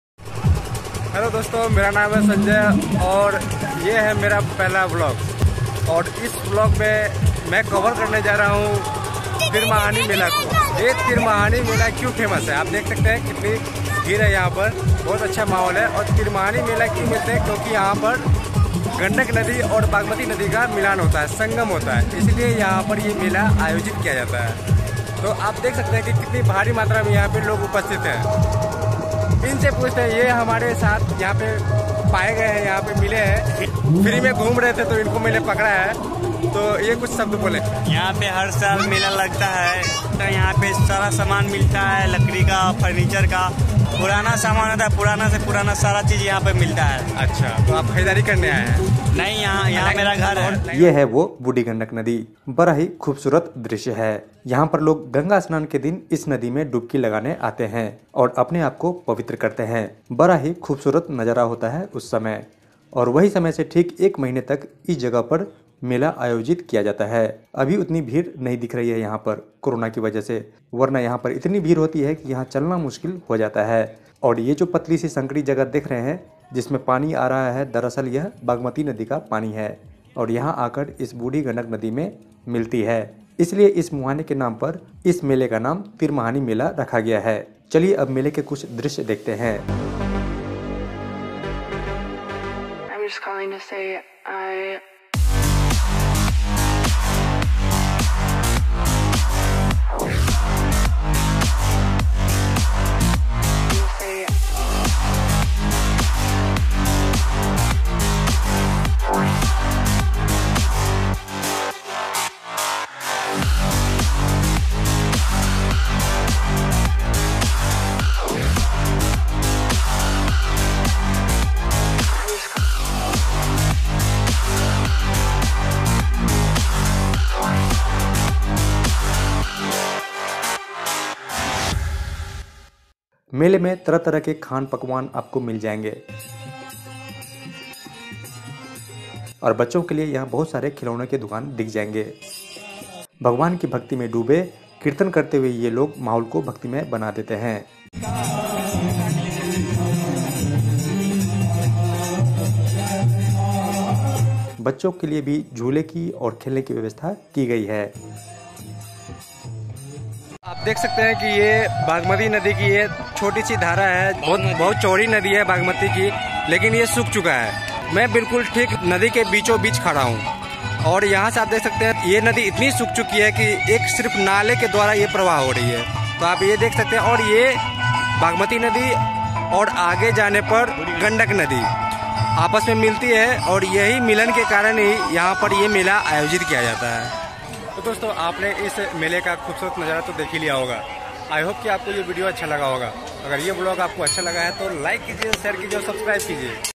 Tirmahani Mela samastipur bihar || my first vlog🔥🔥
hey guys is video me maine tirmahani mela samastipur bihar ko cover kiya hai aur ise vlog ke roop me present kiya hai. ye mera pehla vlog video hai so maine masti masti me video shoot kiya hai. is video me maine tirmahani mela ke kuchh drishyon ko qaid kiya hai aur vlog ke roop me present kiya hai so agar aapko ye video pasand aye to like jaroor karen. thnx